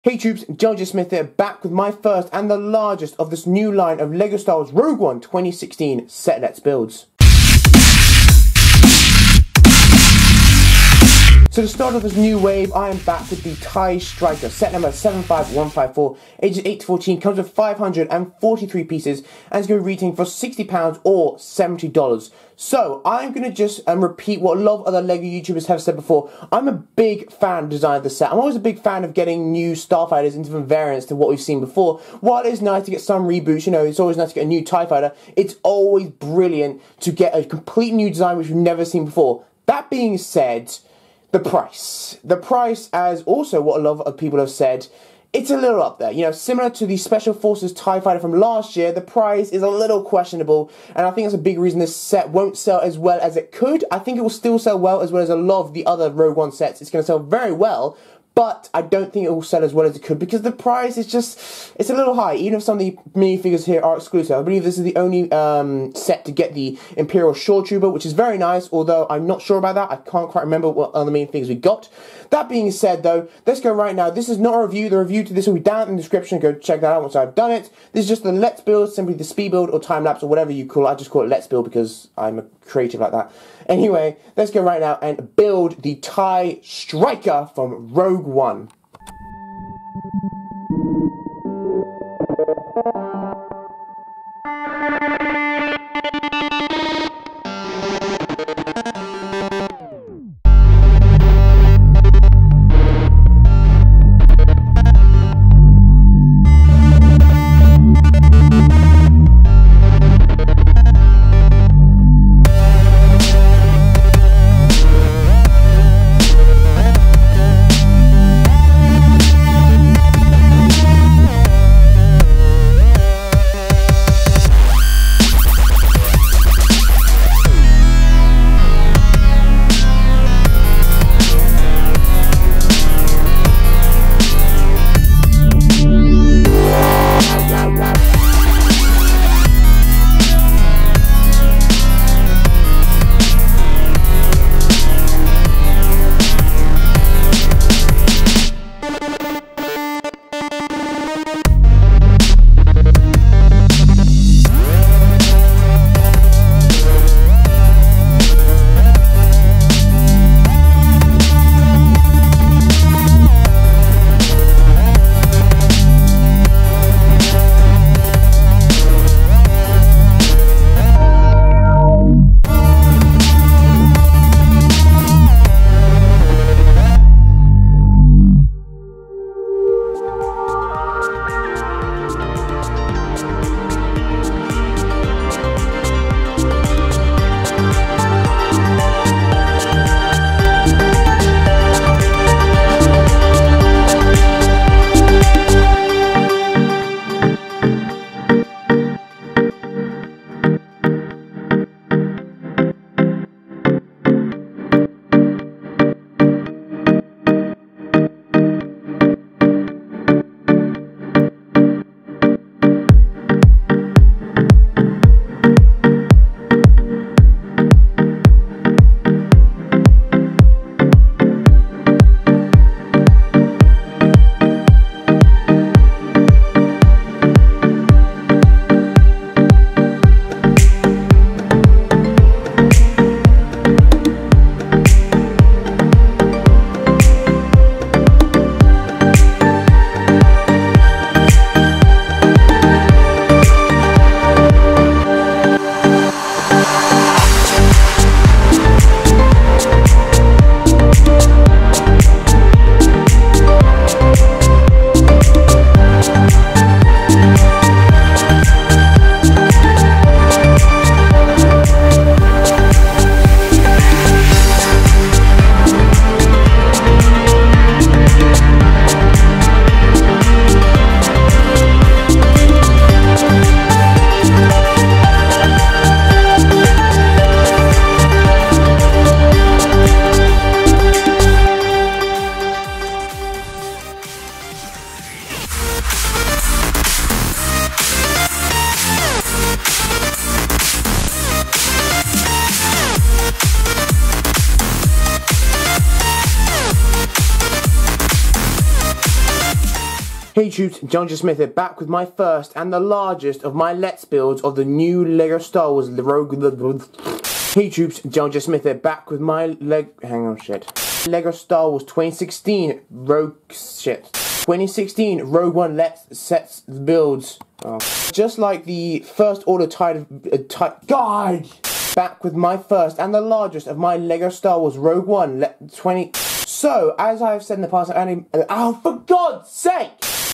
Lego Star Wars Let's Build 20X: TIE Striker 75154
This is the thirty-sixth instalment of my "Let's Build" series and this one is on the TIE Striker 75154
Ages 8-14
543 pieces
£59.99     $69.99